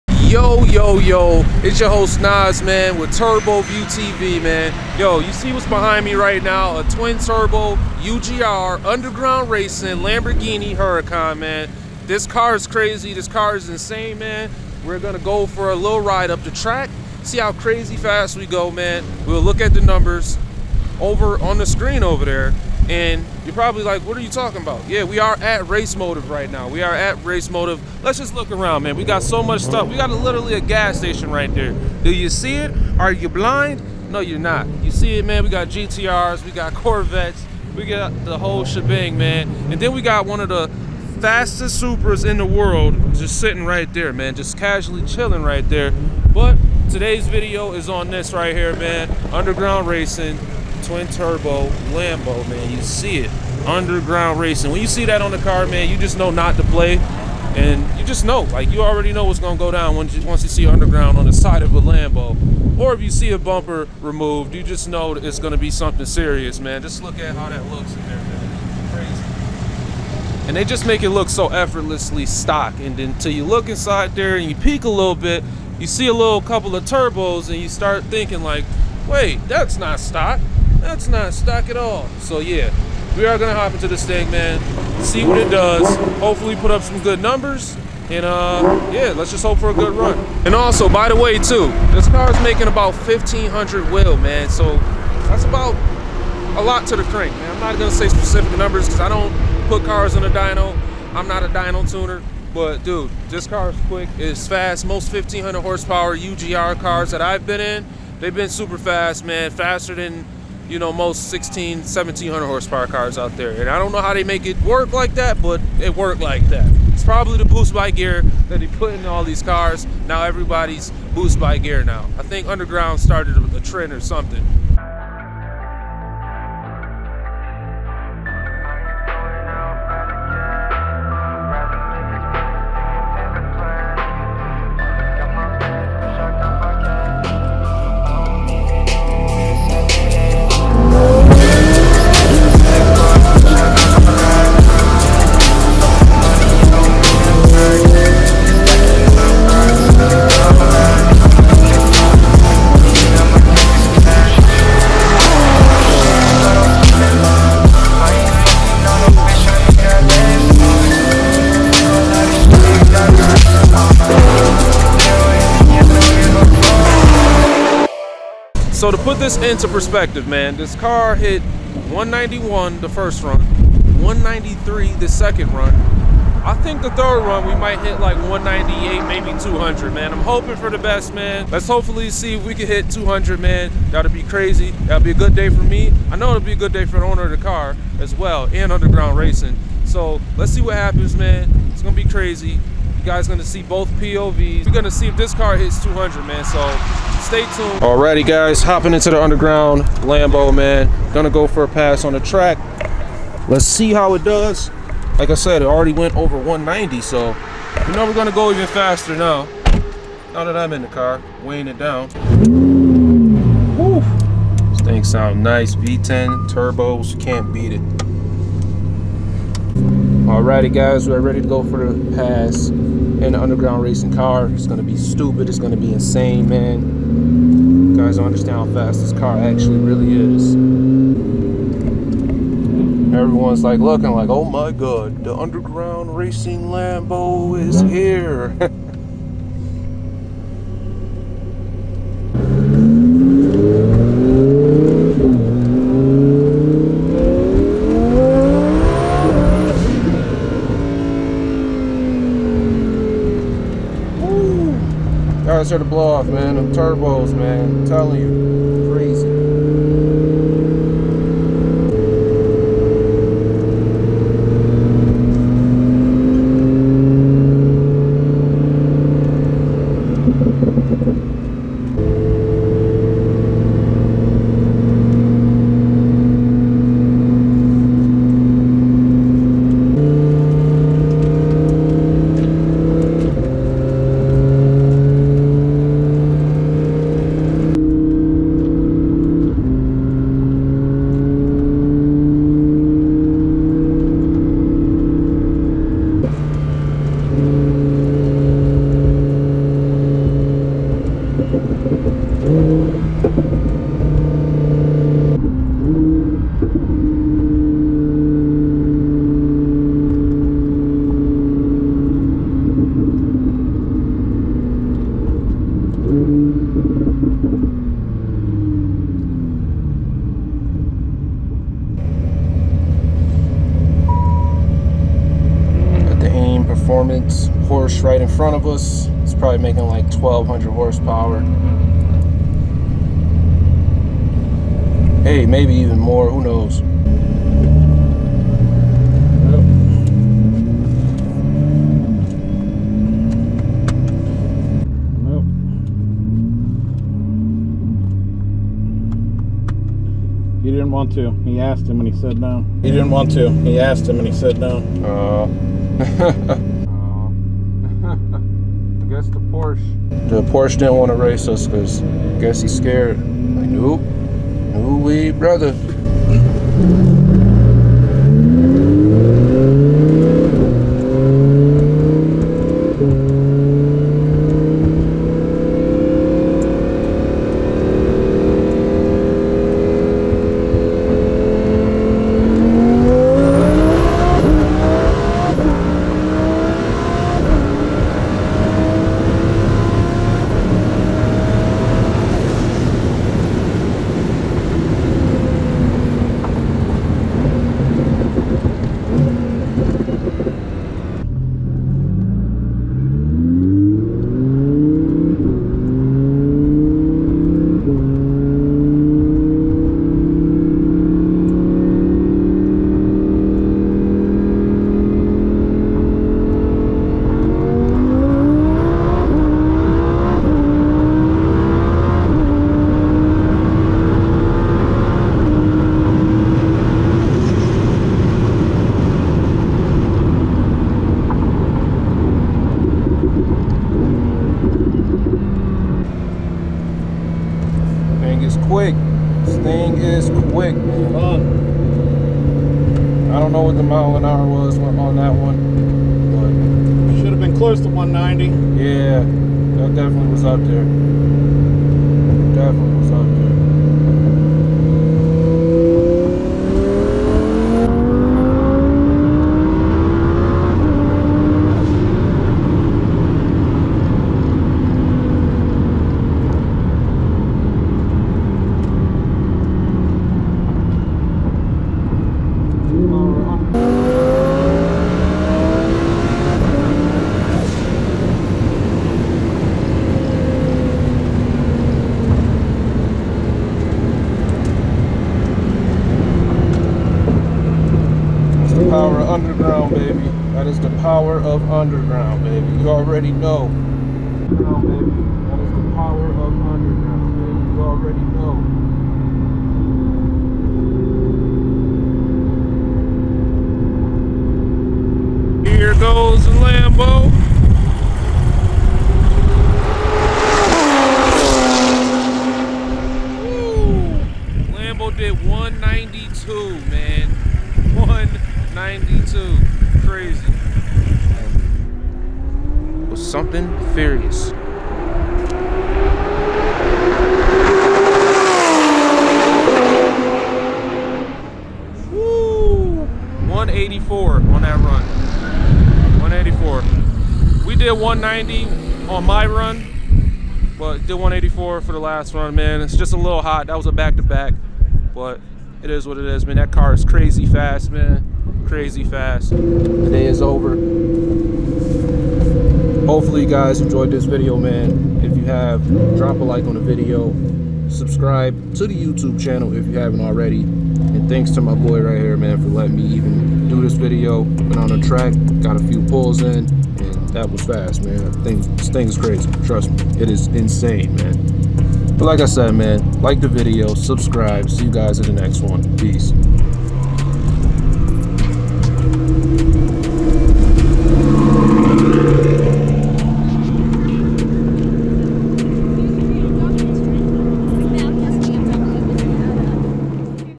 I GO 200MPH DOWN THE POCONO RACEWAY IN A 1500HP HURACAN 😵‍💫
Check out @Turboview_tv on Instagram where i post all the behind the scenes content 💯😎